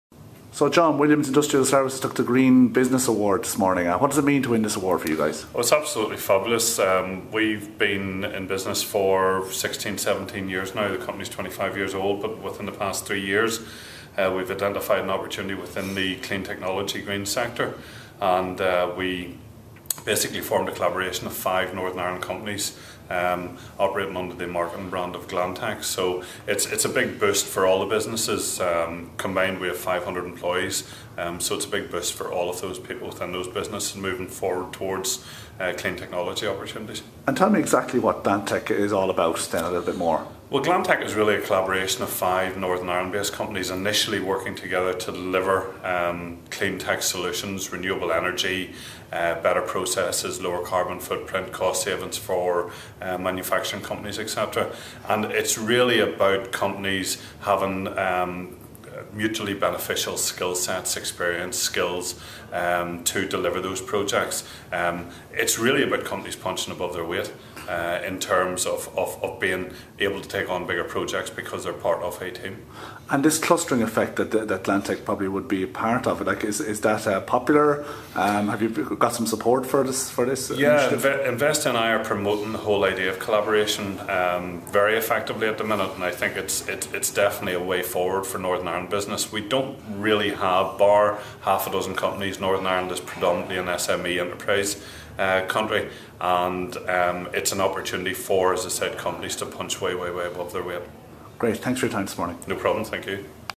Business achiever Ulster; Glantek
Williams Industrial Services is the lead partner company in the Glantek Alliance. Glantek is an unique collaboration of five NI companies, working together to develop and market clean ("green") technology products and processes for waste, waste water and renewable energy.